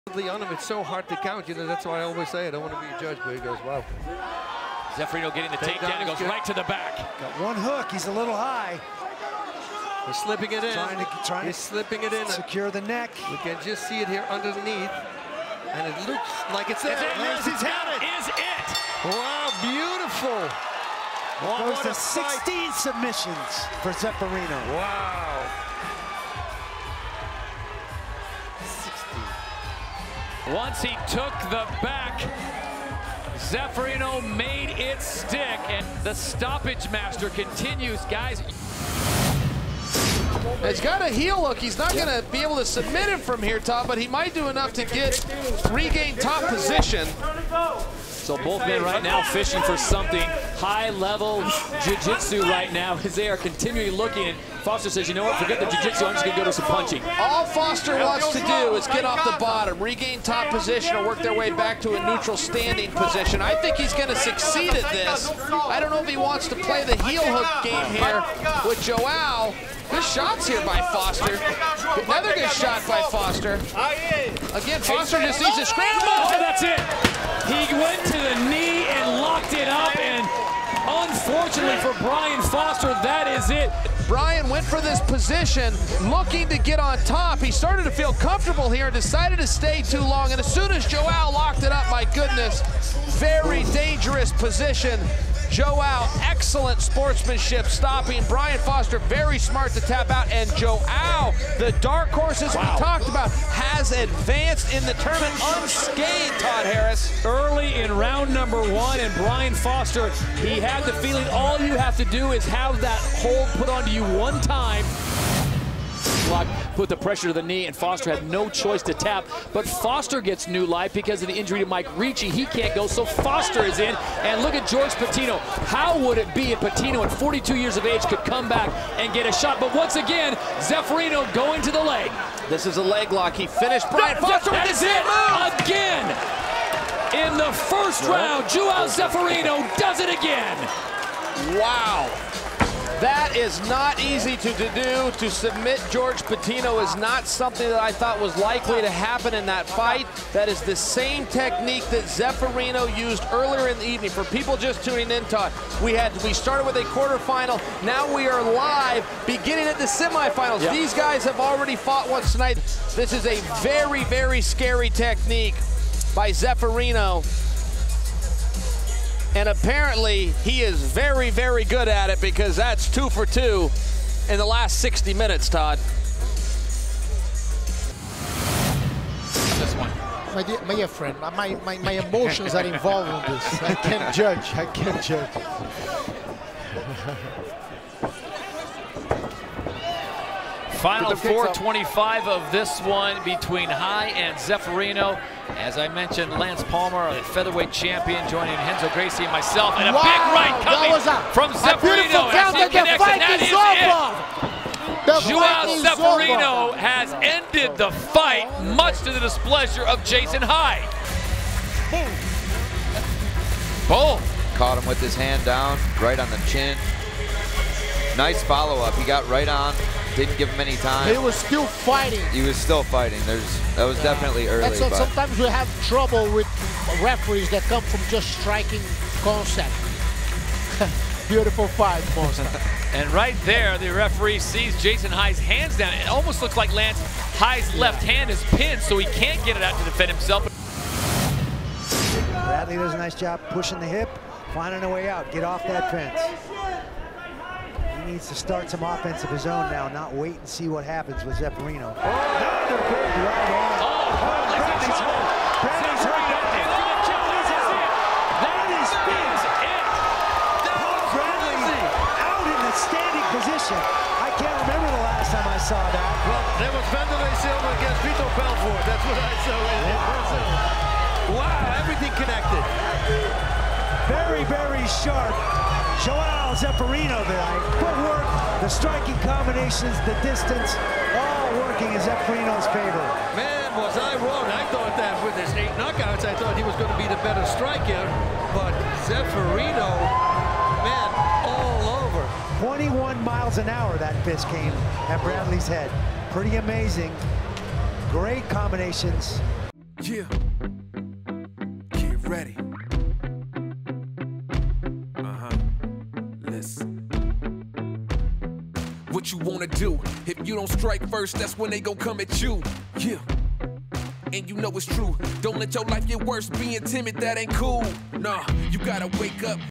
Every João Zeferino Finish in PFL & WSOF
João Zeferino has fought tough opponents during his tenure with PFL & WSOF. Relive every finish from the "Brazilian Samurai."